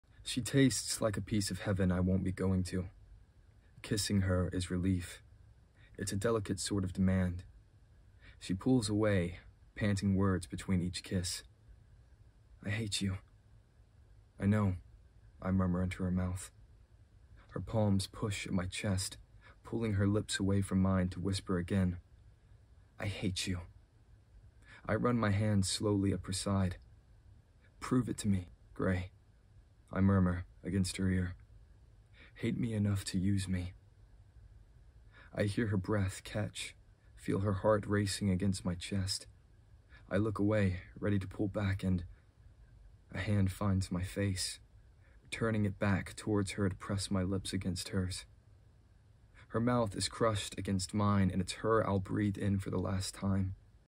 Chase Brown narrates RECKLESS
Watch Chase Brown, the voice of Kai, narrate an excerpt from RECKLESS by #1 New York Times bestselling author Lauren Roberts.

Paedyn and Kai battle with duty and desire in this highly anticipated second installment in the sizzling and epic romantic fantasy trilogy that’s packed with spicy tension and edge-of-your-seat betrayal.